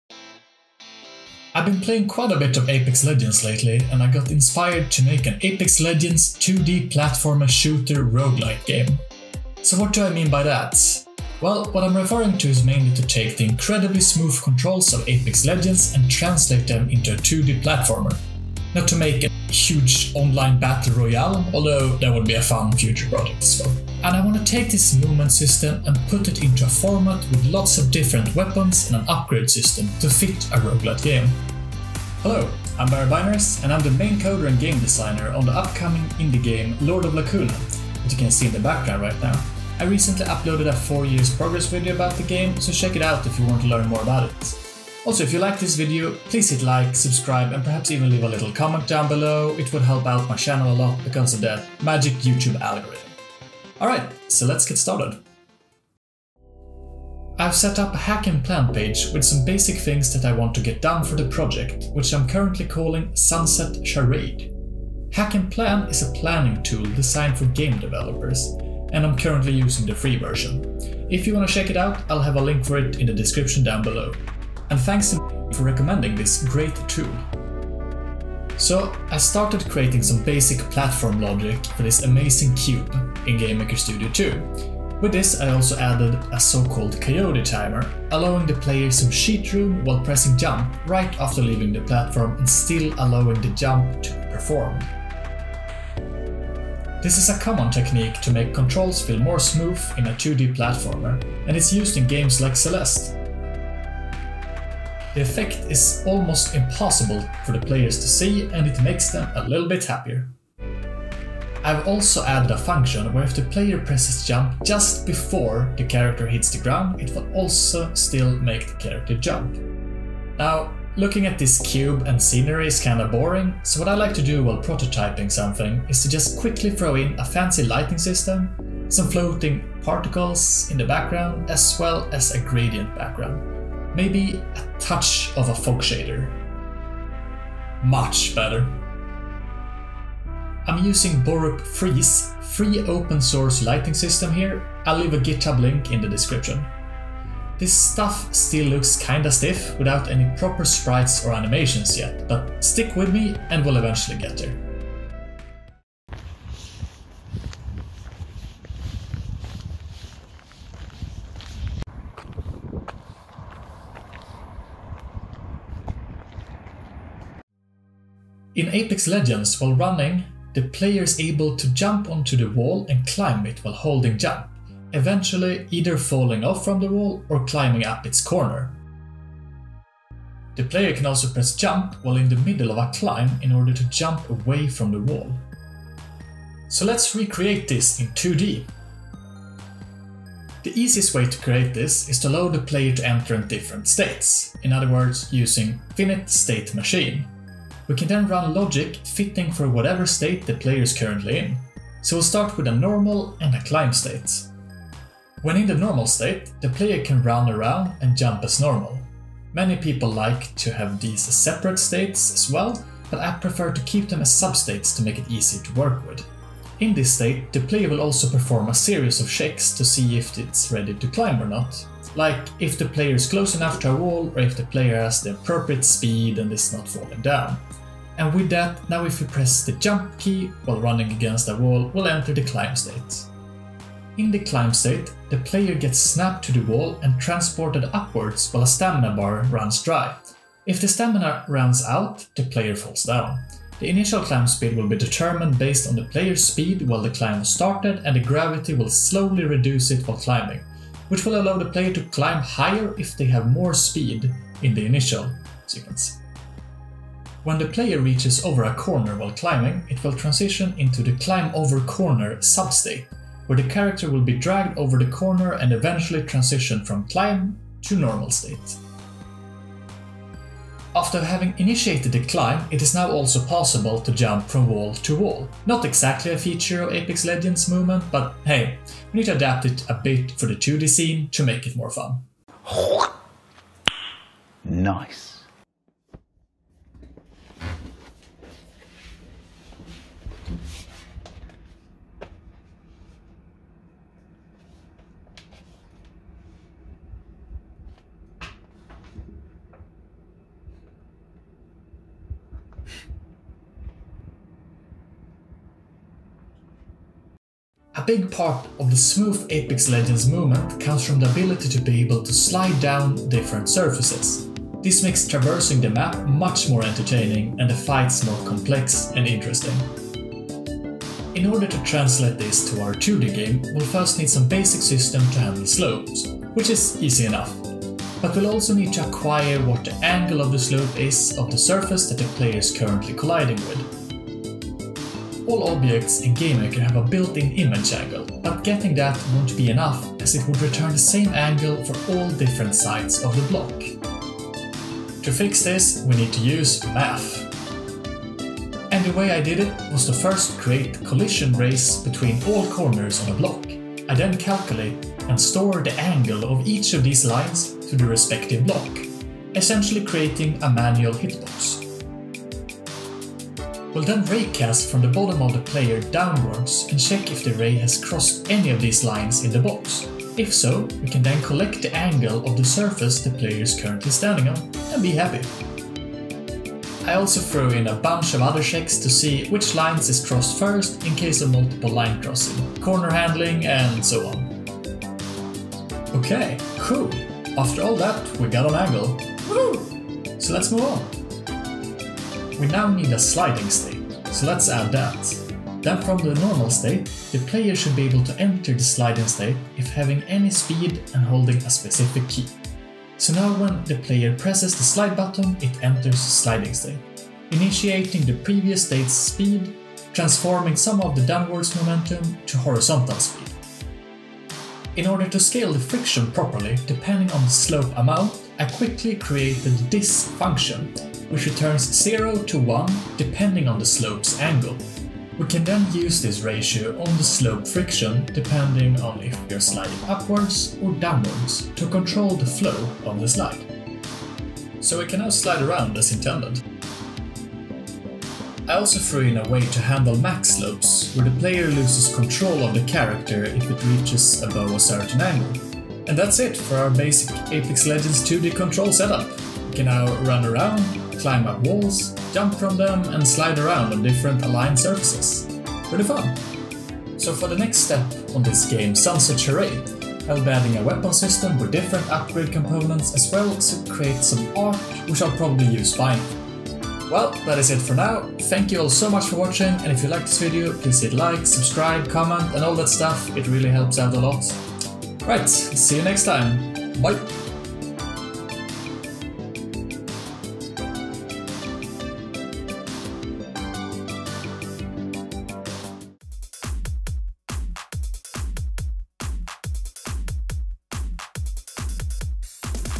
Making Apex Legends movement in 2D - Semi tutorial
I’ve been playing quite a bit of Apex Legends lately and got inspired to make an “apex legends 2d platform shooter roguelite” game.So what do I mean by that? Well what I’m referring to is mainly to take the incredible smooth controls of Apex Legends and translate them into a 2d platformer. Not to make a huge online battle royale (that might be a fun future project though!..).And I want to take this movement system and put it into a format with lots of different weapons and an upgrade system to fit a rogue lite game.